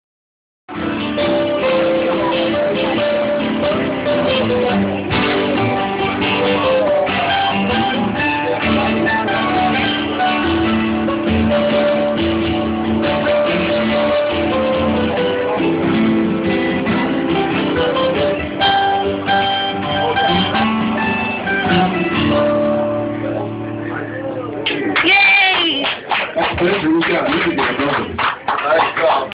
Spencer enjoys the band.
Spencer cheers on his daddy and two of his daddy's friends.